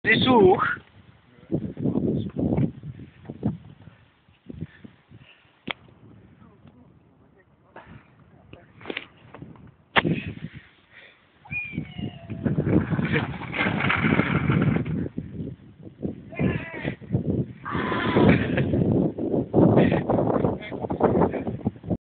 Sjeele Ft. Rolor
Nuudig:
1 Rolor
1 Zondag
3 keender
En een dossis meligheid

En mit als resultaat zoeget wie dit filmke..

Special Thanks To:
Rob veur ut filmen vaan alles en aog op de rolor gaon..
Alain veur 3 filmkes en de dosis meligheid
Sjeele (iech) veur ut briljante idee en de flauwe shit!!